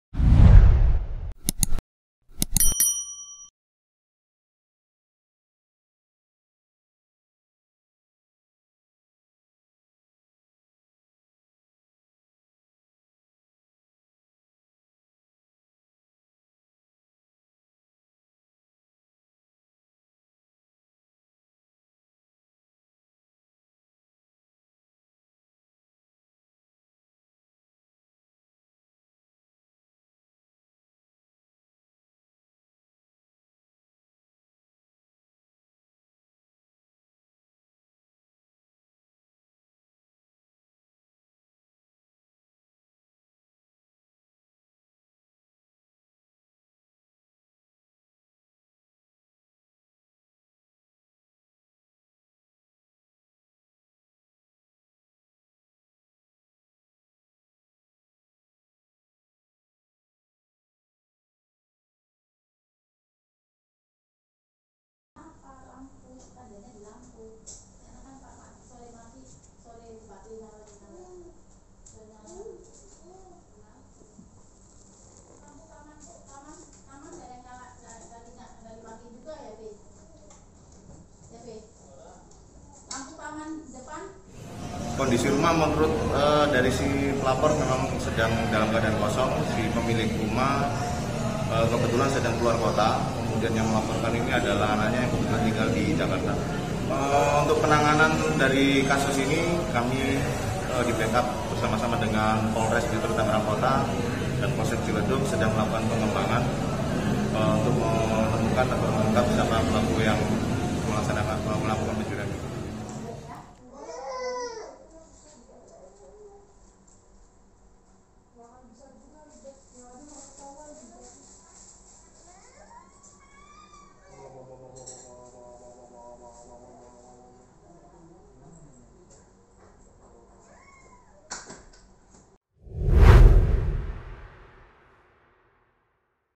RUMAH KOSONG, PERHIASAN DAN 1 MOBIL HONDA JAZZ RAIB
TANGERANG (Pos Kota) - Pelaku pencurian yang terjadi di Komplek Bangun Reksa, Kelurahan Pondok Pucung, Kecamatan Karang Tengah diketahui masuk melalui perkarangan rumah.
Kapolsek Ciledug, Kompol Wisnu Wardhana mengatakan pelaku masuk dengn mencongkel jendela dan langsung menggasak sejumlah barang berharga.
"Pelaku masuk ke rumah pekarangan, kemudian merusak pintu dan mengambil sejumlah perhiasan dan kendaraan roda empat," ujar Wisnu saat dikonfirmasi, Sabtu (26/12/2020).
Wisnu mengatakan, saat kejadian runh dalam keadaan kosong lantaran pemiliknya sedang berada di luar kota.
Menurutnya atas kejadian tersebut kerugian korban ditaksir sekitar Rp300 juta.